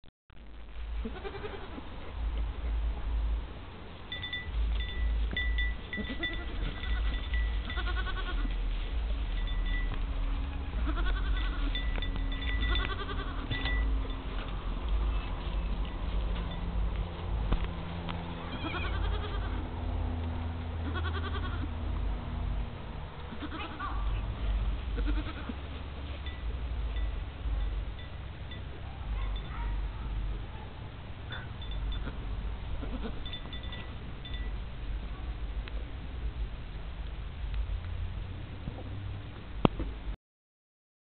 Ralph our Great Pyrenees taking care of his goats
This is Ralph, our 1 year old Great Pyrenees, taking care of, Abby, our one day old pygmy goat. The white goat at the end is Belle, the mama goat. e is so motherly with the baby, so sweet.